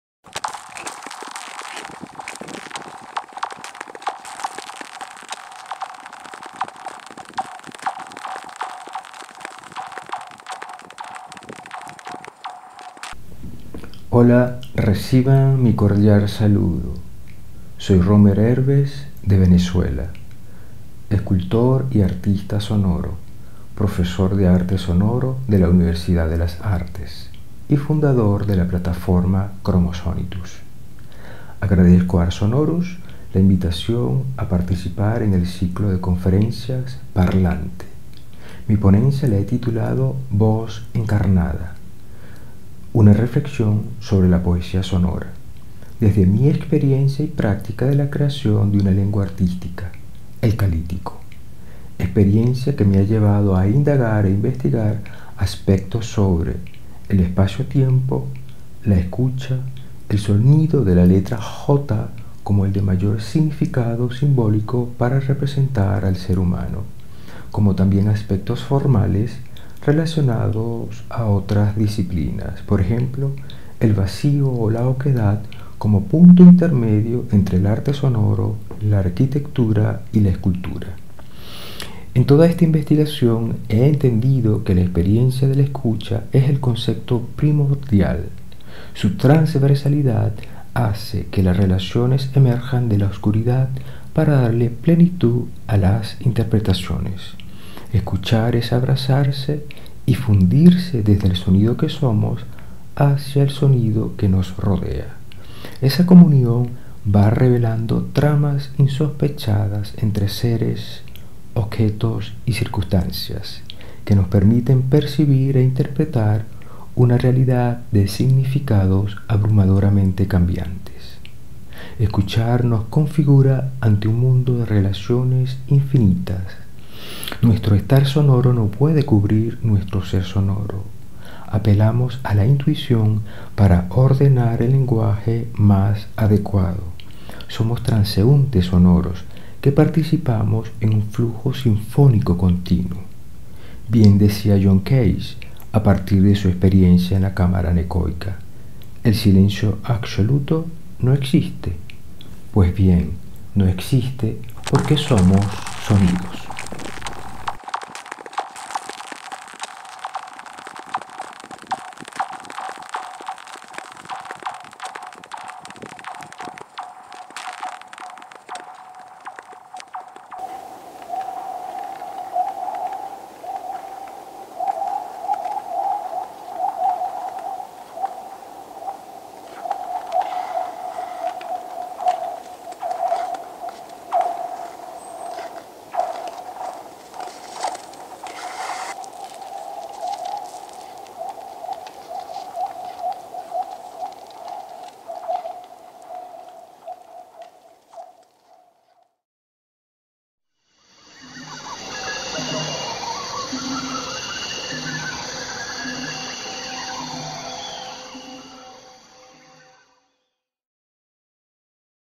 Presentacion Conferencia: Voz Encarnada | Rommel Hervez | Ars Sonorus
Las reflexiones del siguiente ensayo parten de la necesidad de abordar “el instrumento vocal” como una experiencia de relaciones que configura a la persona consigo misma y con su entorno. La voz como instrumento de poder que humaniza si se le entiende como tal. Partiendo de referencias teológicas de Gen. 2, 19-20, se interpreta la participación del ser humano como “voz encarnada”, co- creador, dando nombre a la creación. Participación que se ejerce desde el acto mismo de escuchar.